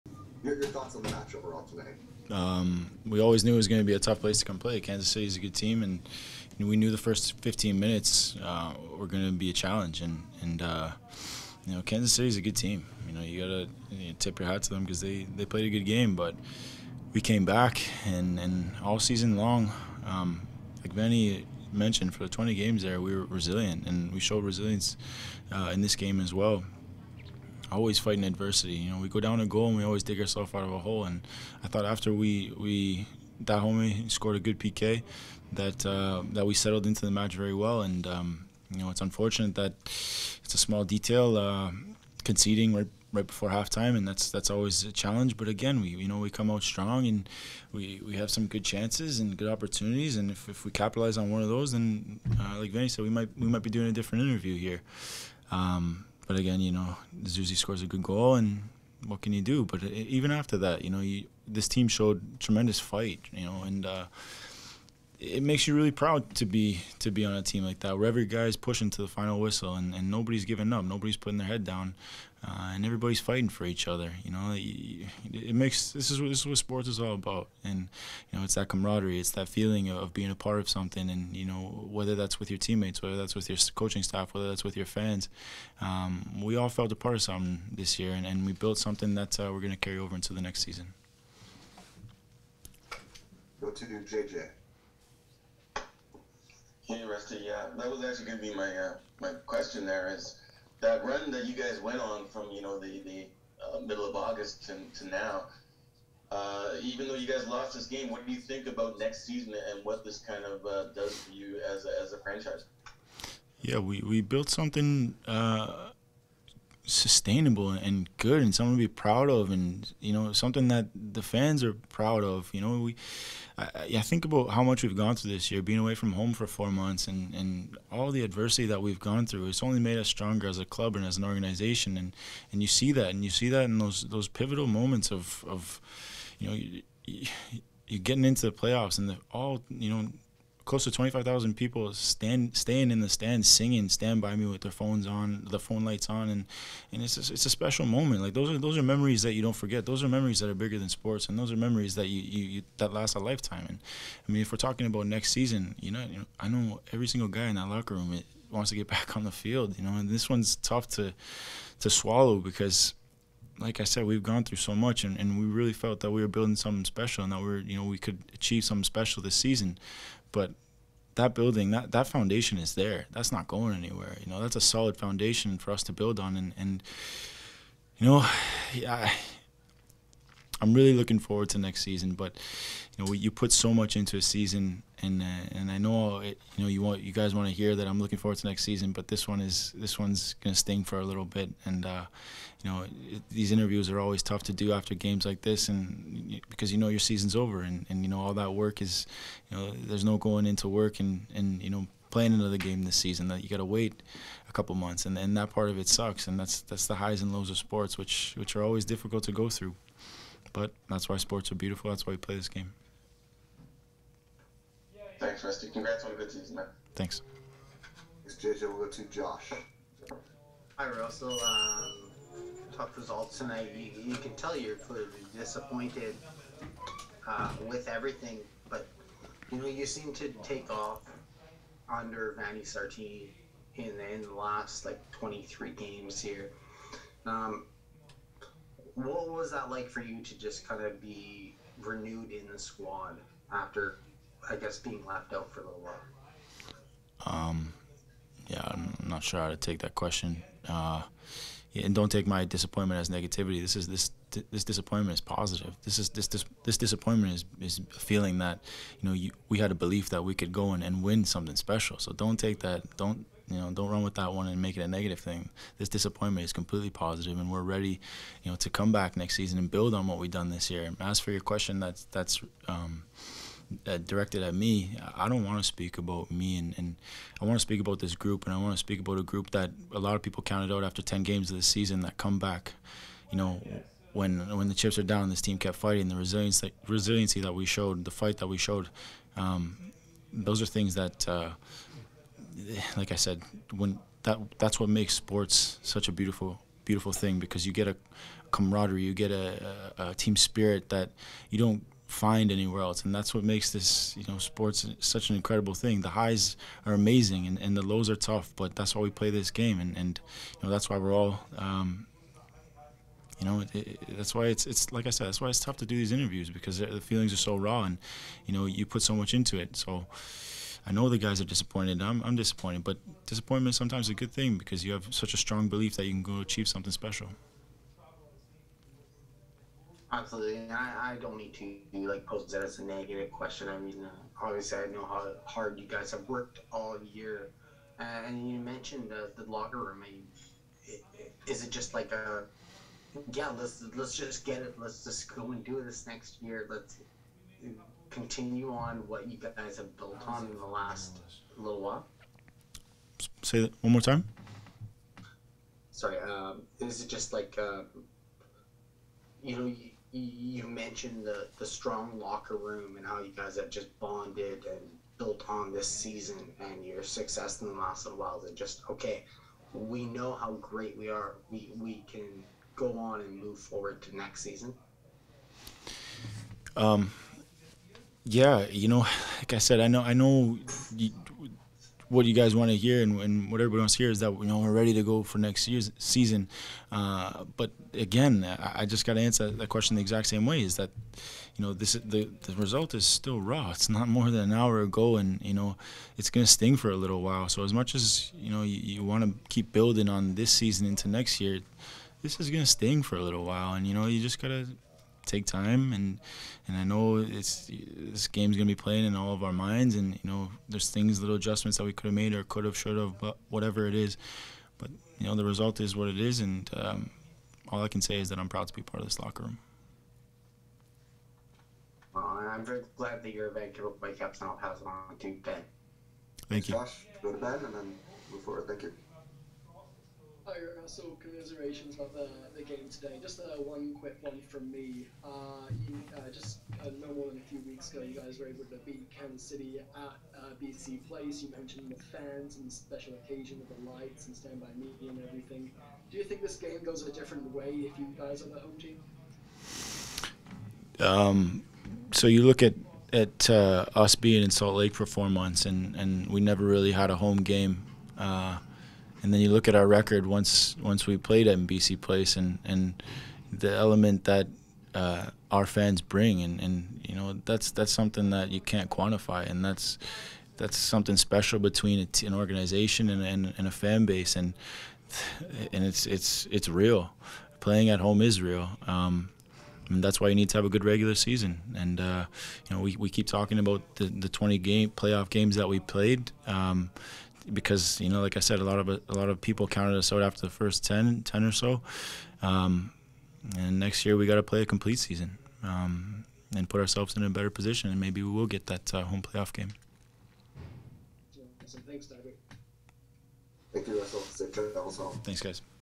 Post-Match: Russell Teibert
Vancouver Whitecaps captain Russell Teibert speak after the MLS Playoff defeat against Sporting Kansas City.